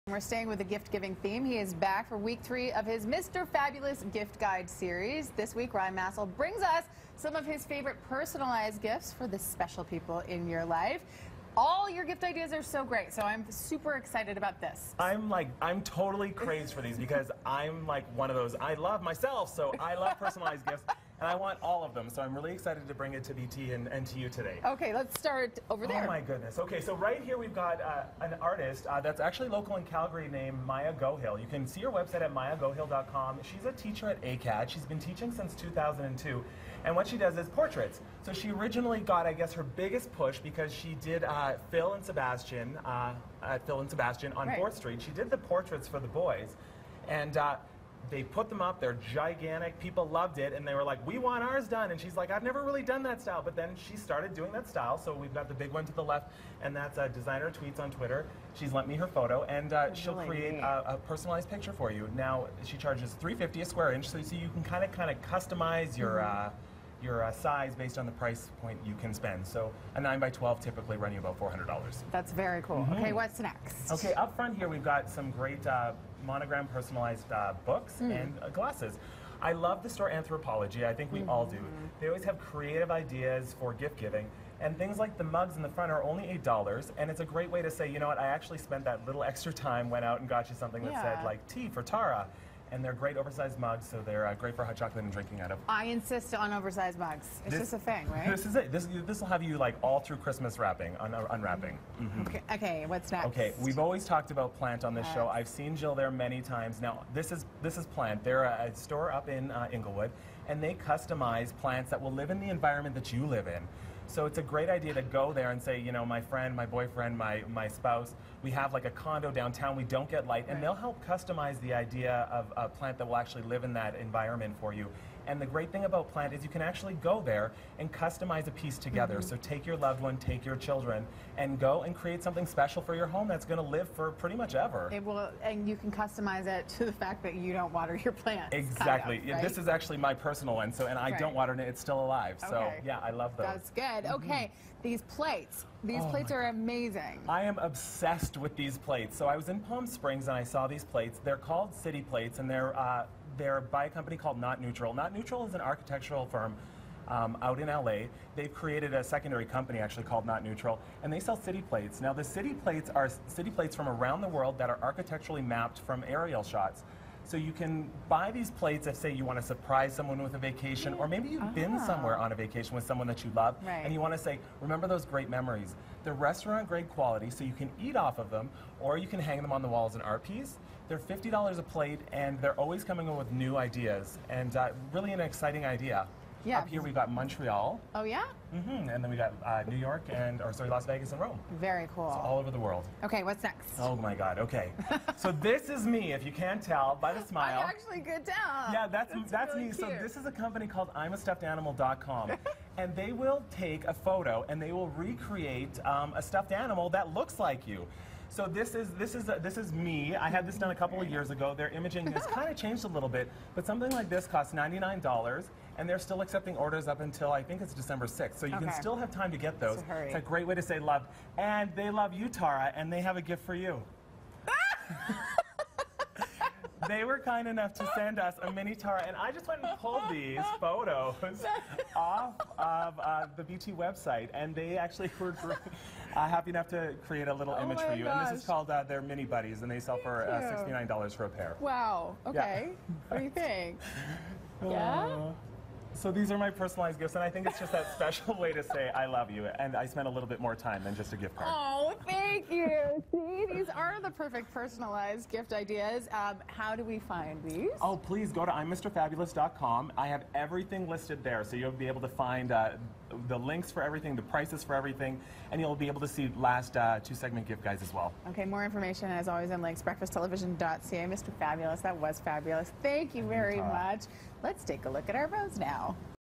Mr. Fabulous - November 27th
Ryan Massel -- A.K.A. Mr. Fabulous -- has his picks for the perfect personalized gift!